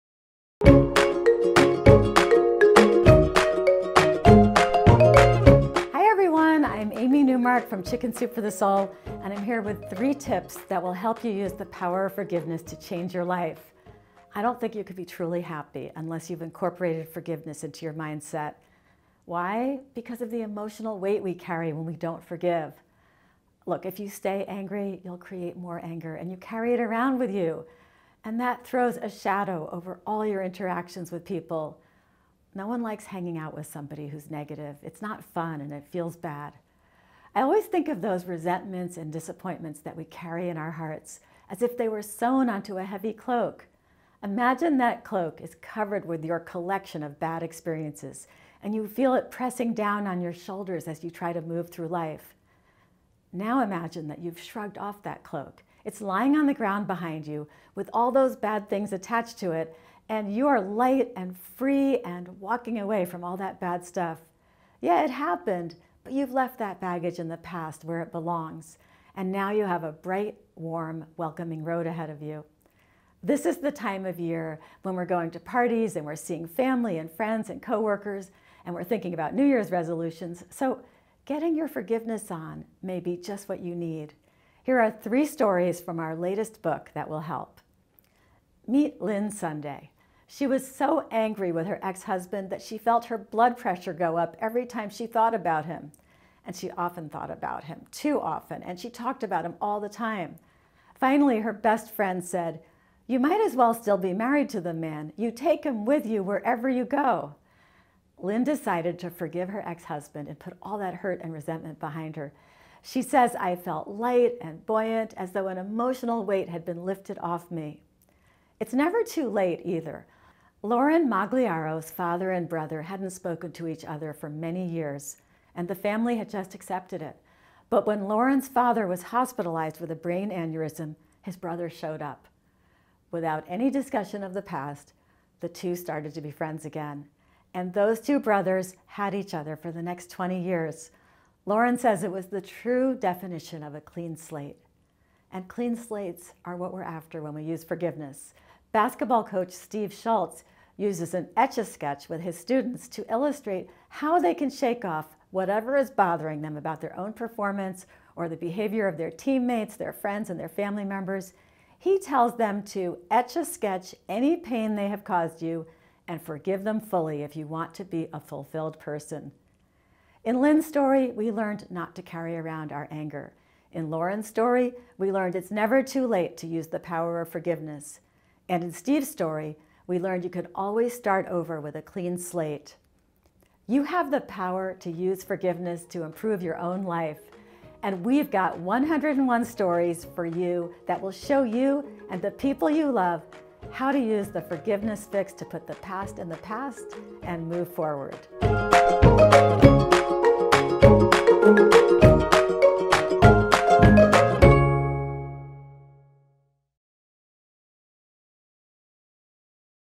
Chicken Soup for the Soul: The Forgiveness Fix
Look beyond the hurt and use the power of forgiveness to move forward! Read 101 stories about putting the past in the past and moving forward in Chicken Soup for the Soul: The Forgiveness Fix.